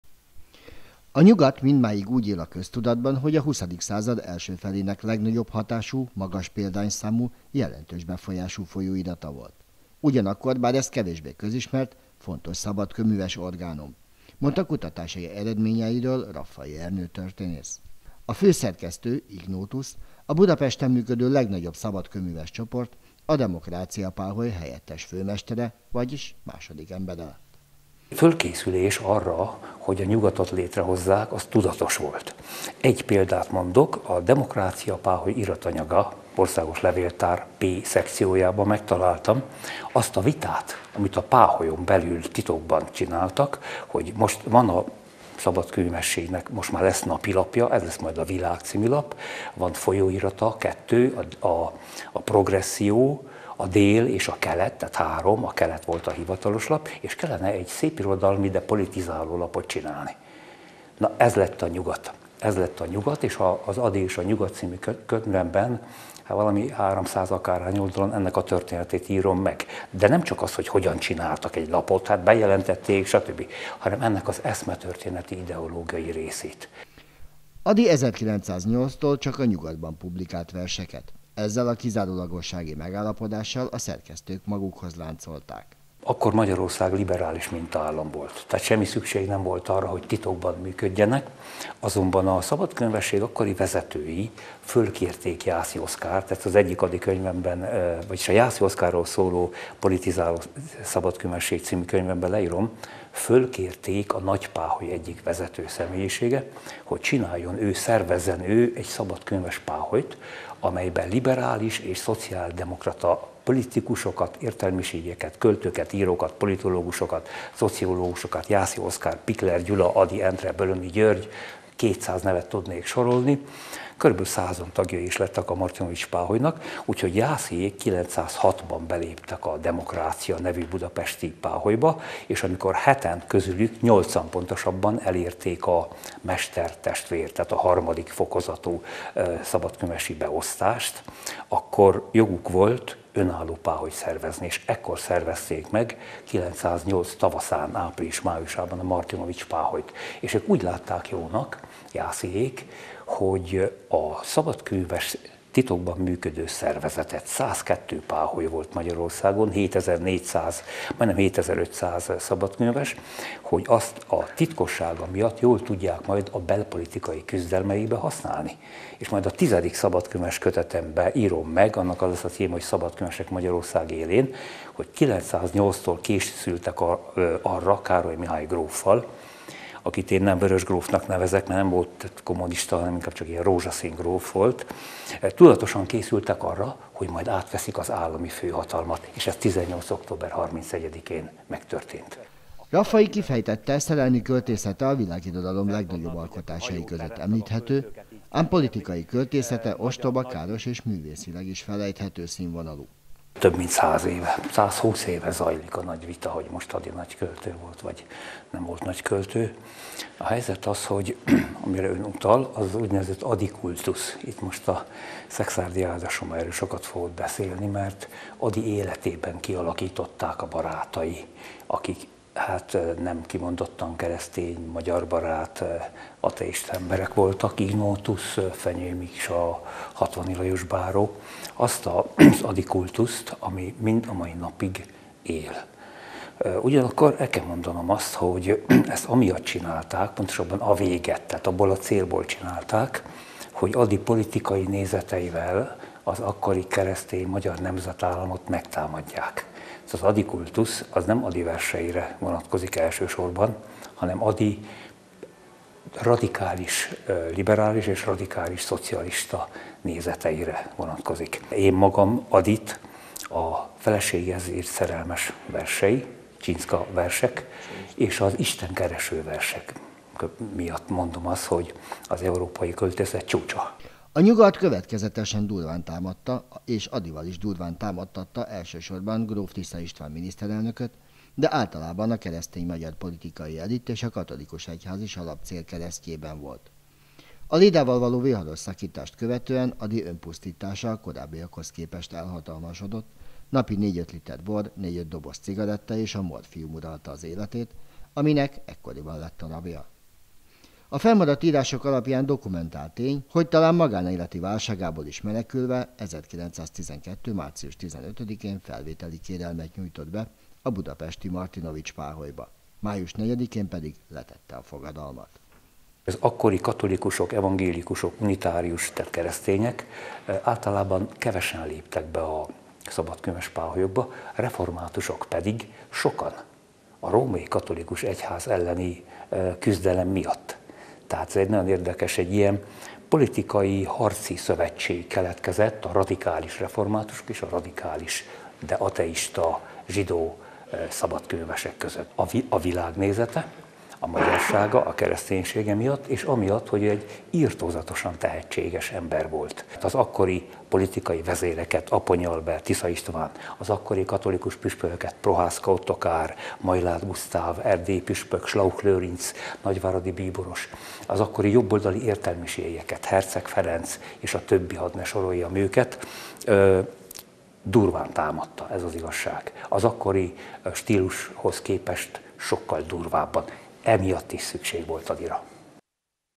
Raffay Ernő történész - szabad kőműveség, Ady Endre
A Vármegyeházi esték sorozatban ezúttal Raffay Ernő történész előadását hallgathatták meg az érdeklődők, aki Ady Endre életének olyan részeire világított rá, mint  a Nyugat című folyóirattal és a szabad kőműveséggel való viszonya. Raffay megítélése szerint mindaz ami Adyt európai szintű költővé tette a szerelmes verseiben és az Isten kereső művekben lelhető fel.  Ám a kor politikai harcaiban résztvevő költészete- Raffay szerint- nem tartozik a művészet magas minőségébe.